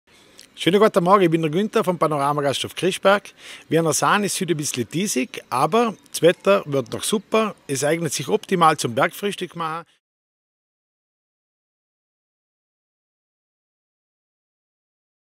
Günter lädt Dich auf das echte Montafoner Bergfrühstück am ...
Günter lädt Dich auf das echte Montafoner Bergfrühstück am Kristberg im Silbertal, dem Genießerberrg im Montafon ein.